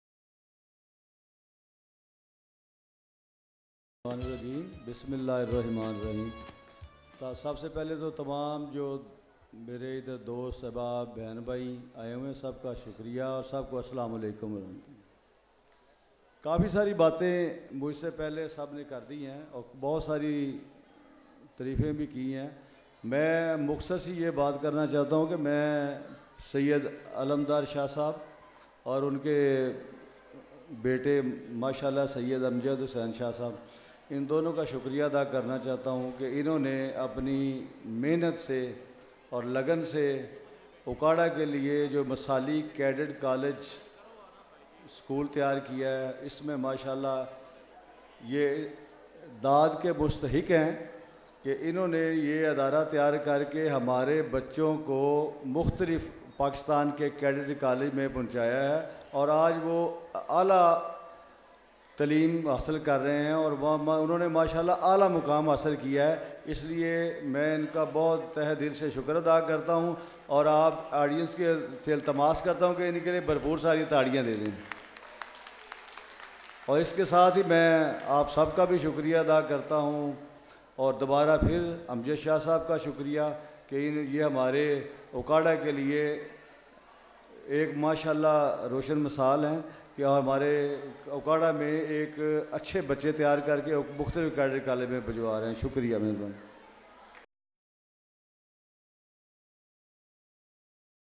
DSP Afzal Khan Speech at Annual Ceremony of the Bahar Cadet School Okara.
🏅Celebrating Achievements: Cadets of Bahar Cadet School honored for their dedication and commitment to excellence.